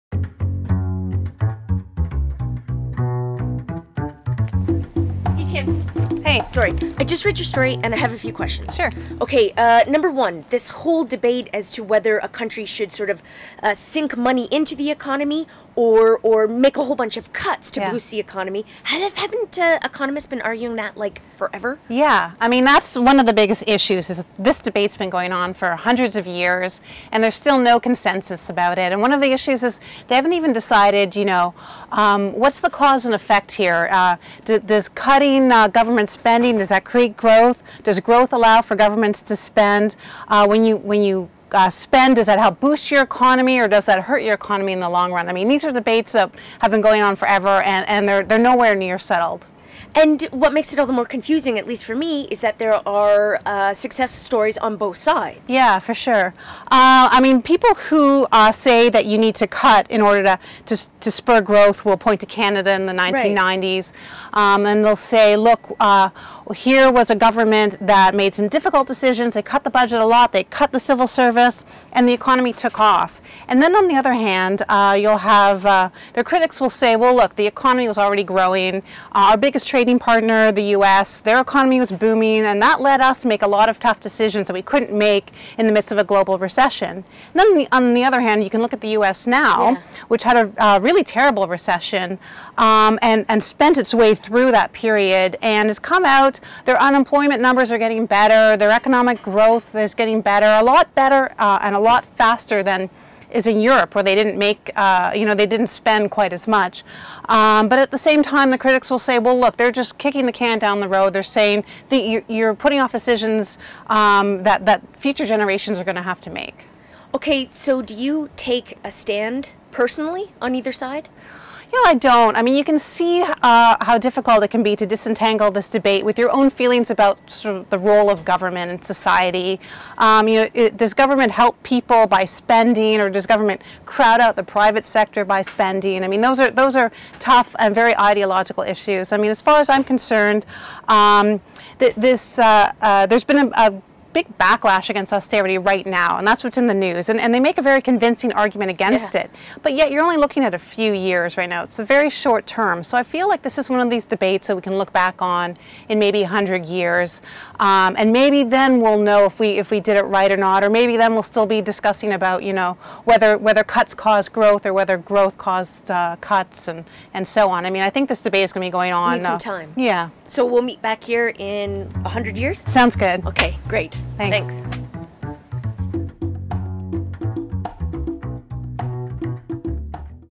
Tamsin McMahon on the worldwide austerity debate
Jessica Allen asks Tamsin McMahon a few questions about her feature on the worldwide austerity debate in the Maclean's library